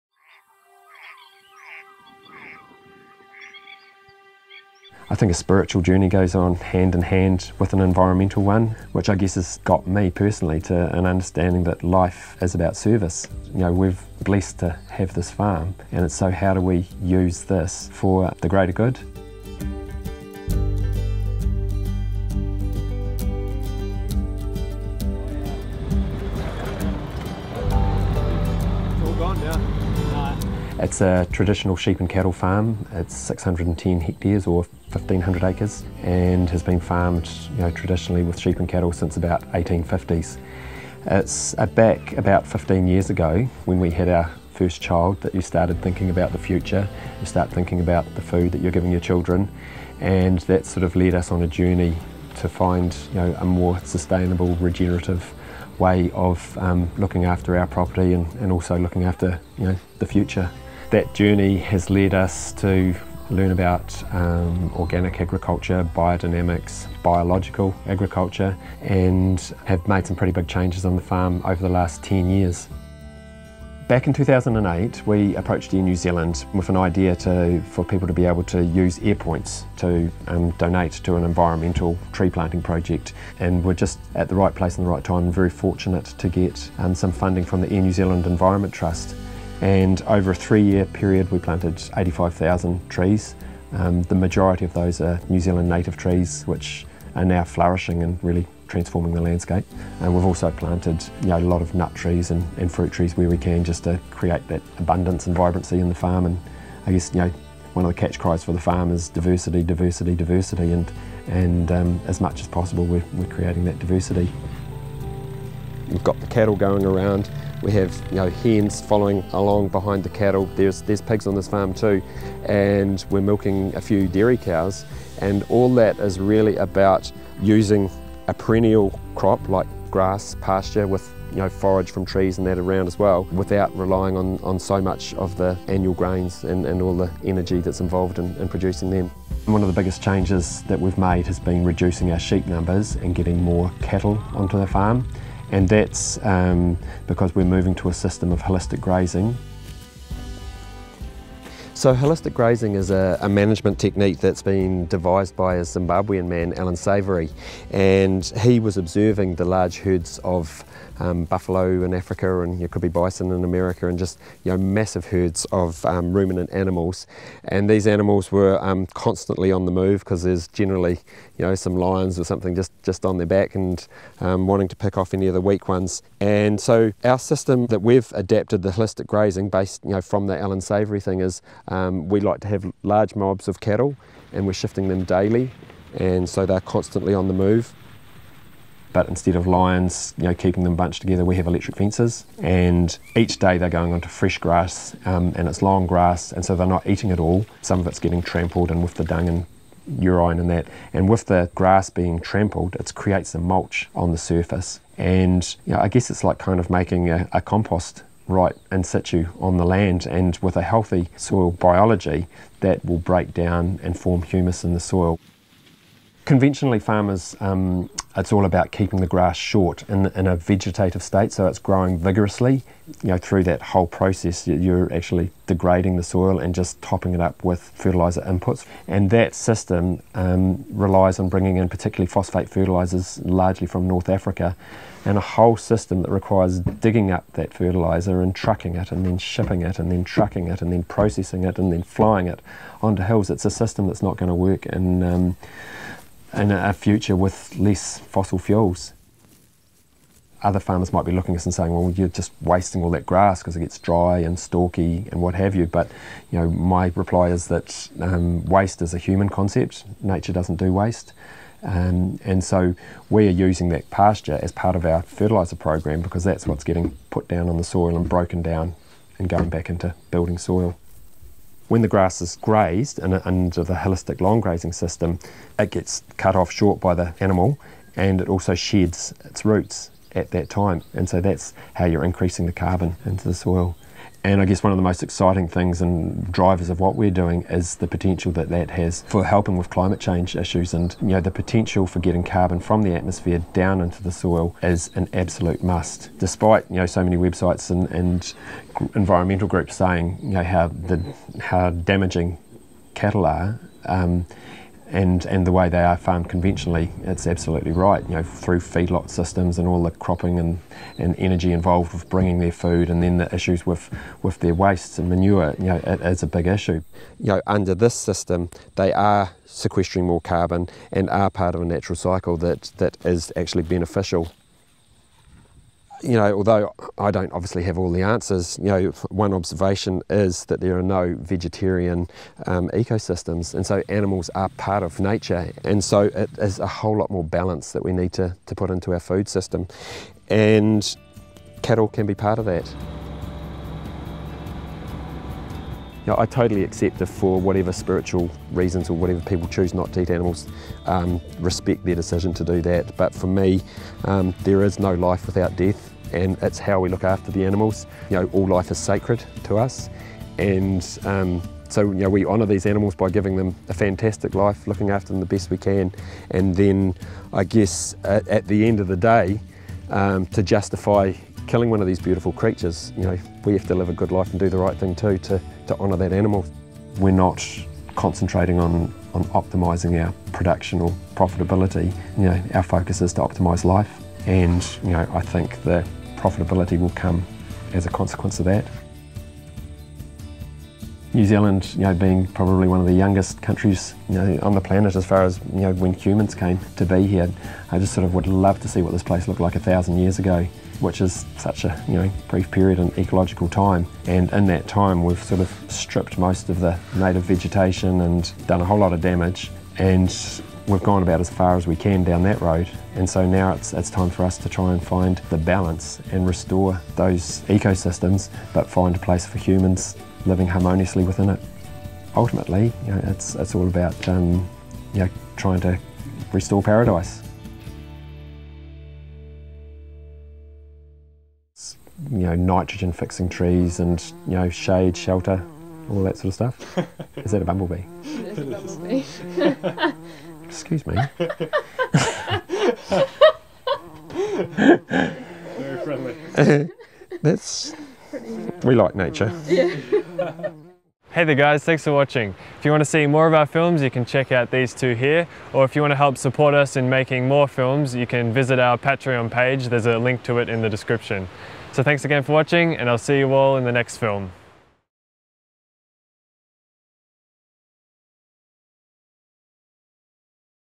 Farming Sustainably with Regenerative Agriculture | Restoring Paradise
Regenerative agriculture offers a future for sustainable farming of meat in line with nature’s needs, by using holistic management and organic/biodynamic practices and even sequestering carbon in the soil – so important in our response to our climate crisis. At Mangarara, in New Zealand’s beautiful Hawke’s Bay, Greg Hart and his family are in the process of restoring 1500 acres of land, conventionally farmed for over 150 years, into the paradise it once was.

** Screen our films in your community! **
While our short films are free to view online, you’ll need to purchase a license to hold a public screening. for info.